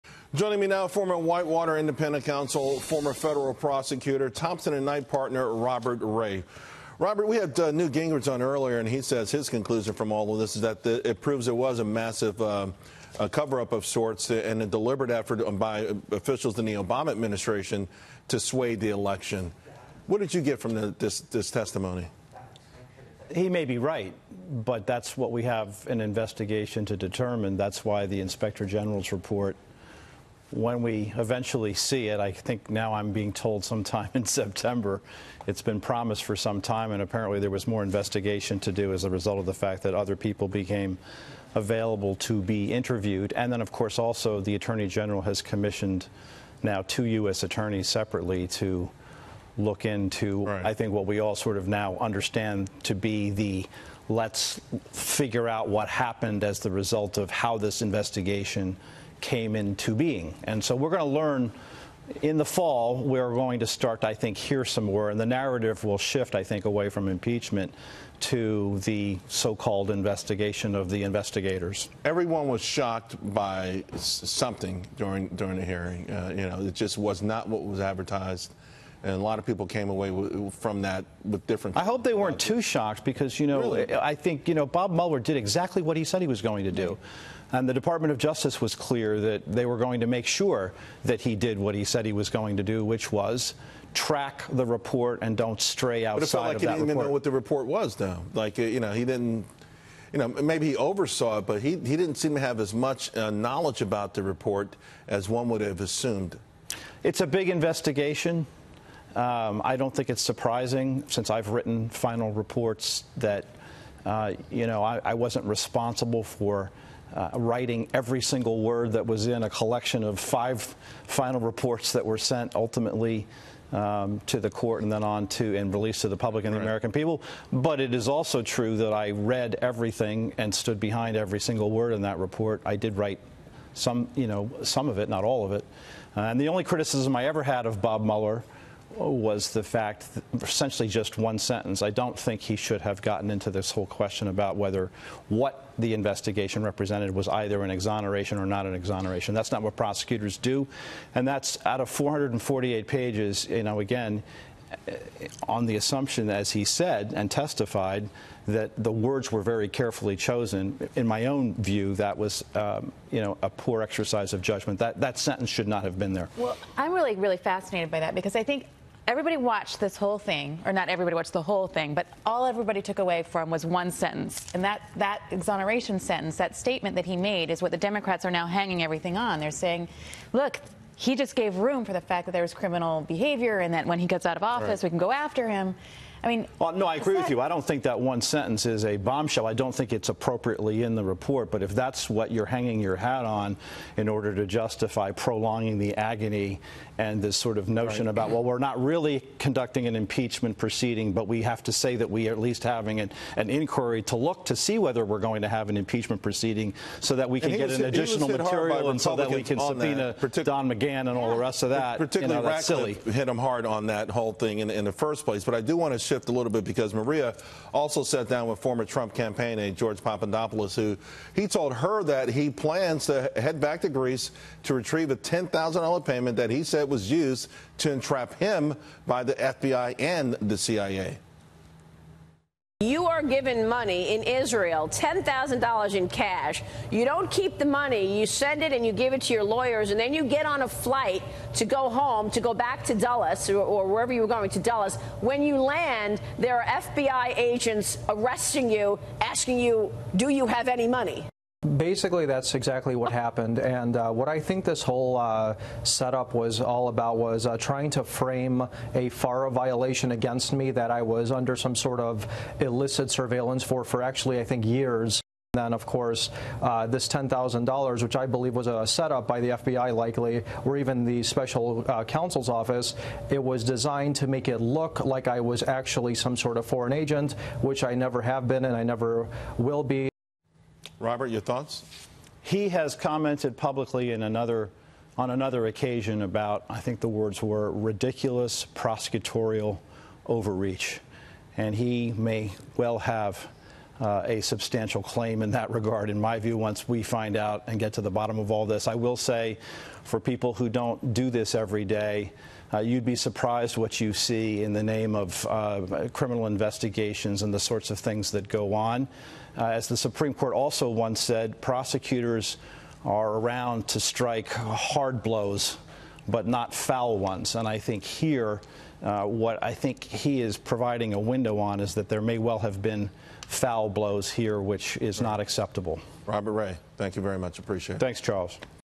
Robert Ray: Mueller did exactly what he said he was going to do
Former Whitewater independent counsel Robert Ray on the Mueller report findings and the future of the investigation.